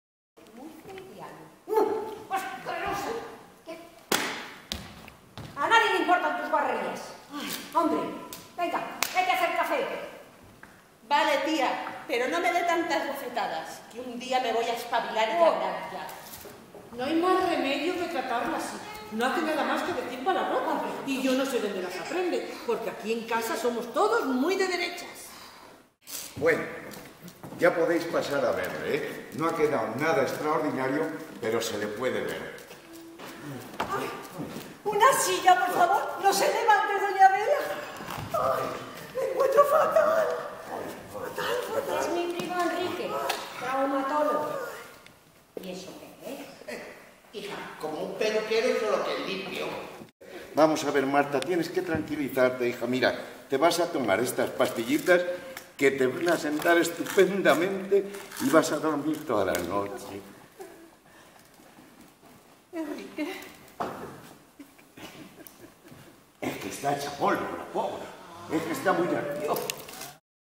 Ensayo del grupo de teatro de Moralzarzal "Tercera Fila"
Ensayo del grupo de teatro de Moralzarzal "Tercera Fila" para su representación de fin del curso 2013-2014.